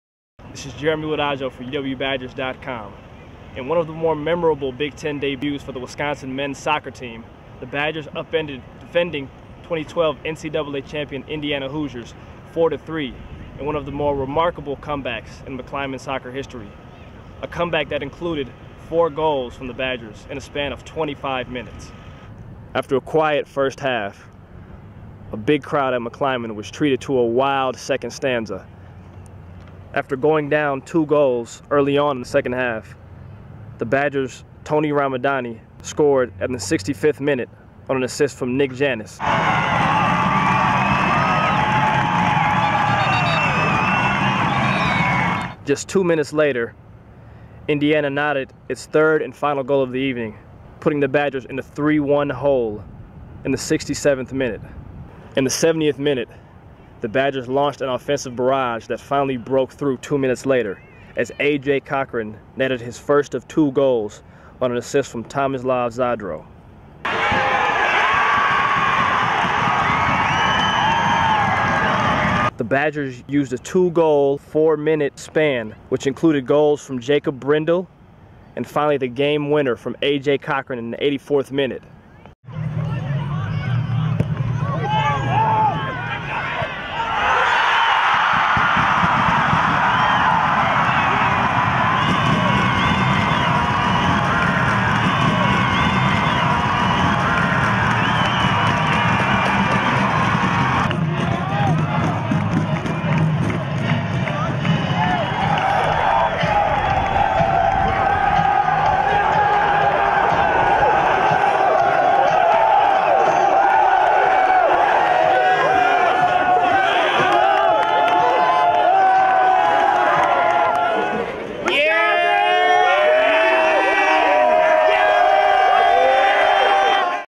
Wisconsin vs Indiana Soccer Highlights
Wisconsin moved to 8-2-1 on the season with a thrilling come from behind victory over Indiana. Jeremy Wodajo recaps all the action from McClimon.